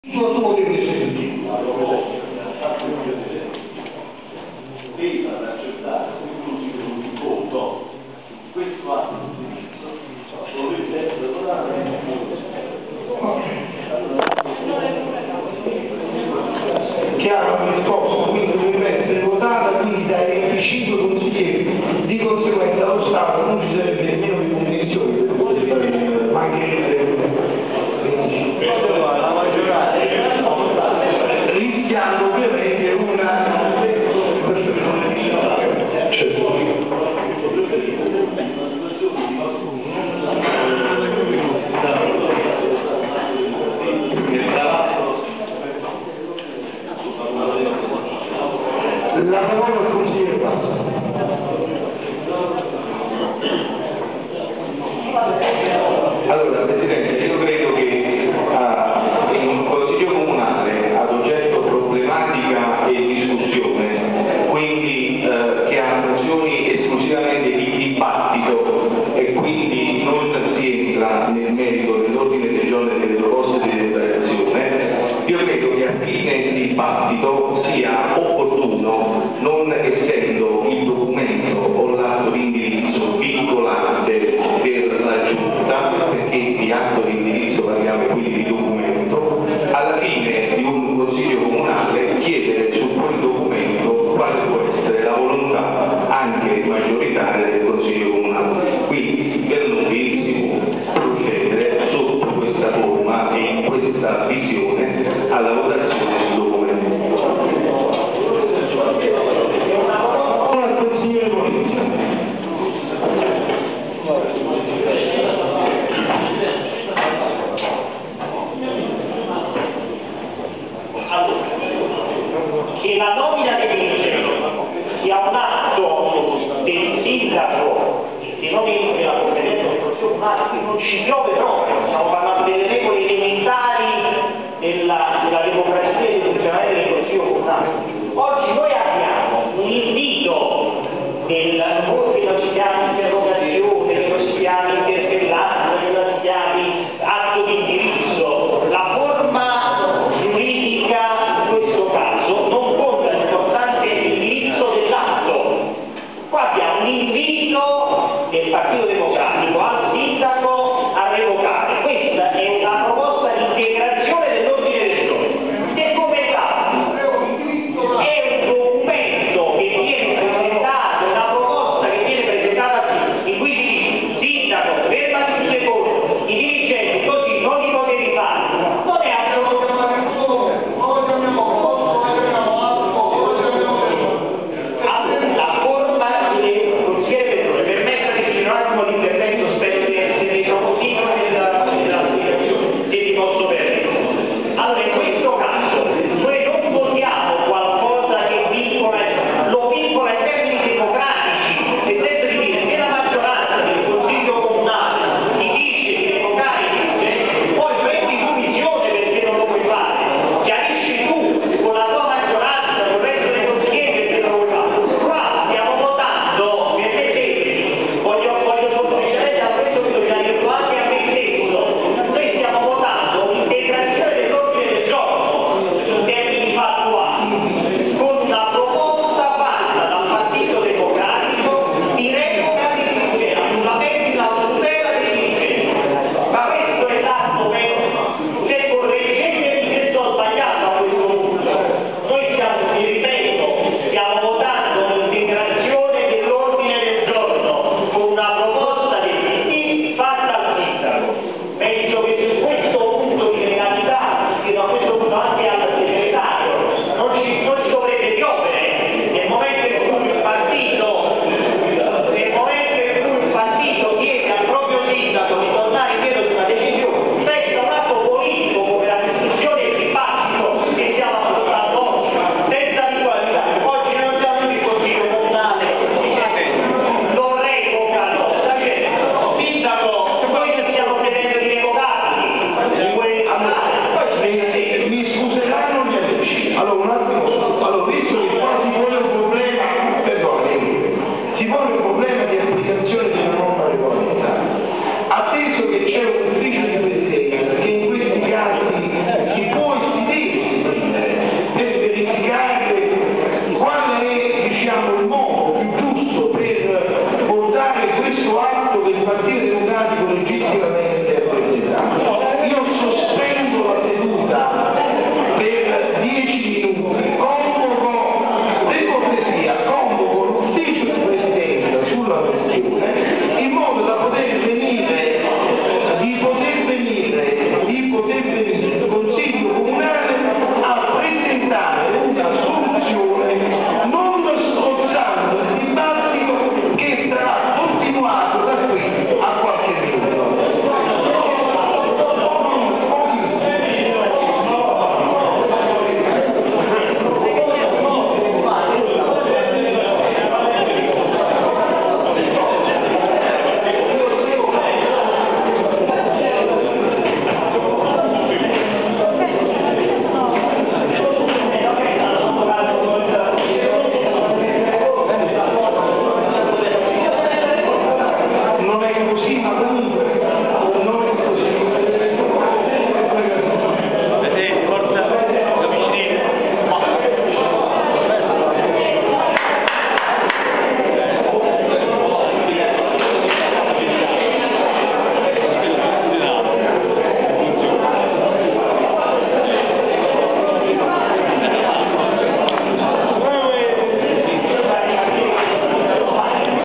CASORIA 17/10/2011 - C. C. "Nomina Dirigenti" (26)
"Casoria Ieri & Oggi online" presenta:
Casoria venerdì 17 ottobre 2011,  ore 13.00, presso la Sala Consiliare, sita in piazza Cirillo, seduta del Consiglio Comunale.
All'ordine del giorno sono fissati i seguenti punti:
Nomina dei Dirigenti, ex art. 110 del Tuel.
Interventi dei consiglieri Giuseppe Balsamo (Pd) e Emilio Polizio (Udc)